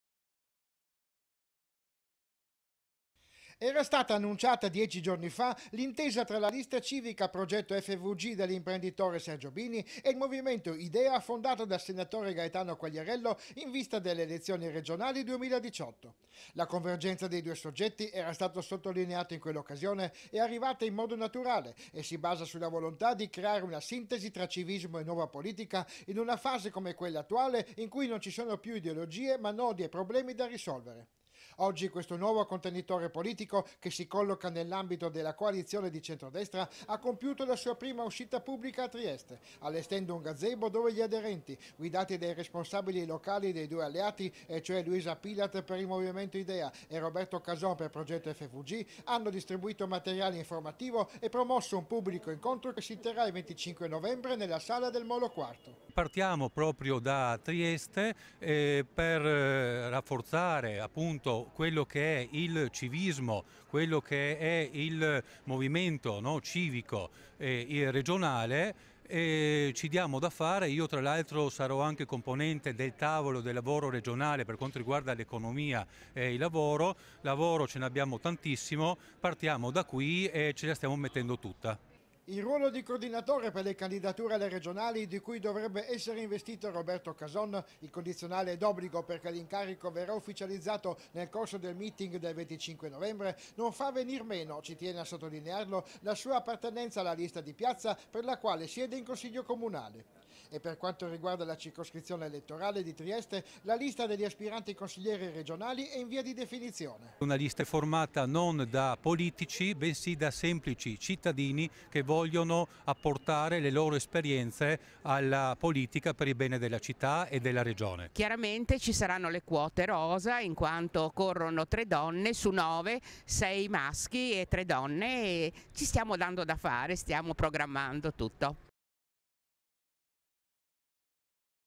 18/11/2017 - IL 25 NOVEMBRE AL MOLO IV L'EVENTO DI PROGETTO FVG E IDEA
IL NOTIZIARIO (venerdì 17 novembre 2017) - Era stata annunciata 10 giorni fa l’intesa tra la lista civica  Progetto FVG” dell’imprenditore Sergio Bini e il Movimento “Idea” fondato dal senatore Gaetano Quagliariello in vista delle elezioni regional 2018.
La convergenza dei due soggetti, era stato sottolineato in quell’occasione, è  arrivata in modo naturale e si basa sulla volontà di creare una sintesi tra civismo e nuova politica, in una fase come quella attuale in cui non ci sono più ideologie, ma nodi e problemi da risolvere.
Oggi questo nuovo contenitore politico che si colloca nell’ambito della coalizione di centrodestra ha compiuto la sua prima uscita pubblica a Trieste, allestendo un gazebo dove gli aderenti, guidati dai responsabili locali dei due alleati, e cioè Luisa Pilat per il movimento Idea e Roberto Cason per progetto FVG, hanno distribuito materiale informativo e promosso un pubblico incontro che si terrà il 25 novembre nella sala del Molo IV
Il ruolo di coordinatore per le candidature alle regionali di cui dovrebbe essere stato investito Roberto Cason, il condizionale è d’obbligo perché l’incarico verrà ufficializzato nel corso del meeting del 25 novembre, non fa venir meno, ci tiene a sottolinearlo, la sua appartenenza alla Lista Dipiazza per la quale siede in Consiglio comunale.
E per quanto riguarda la circoscrizione elettorale, la lista degli aspiranti consiglieri regionali è in via di definizione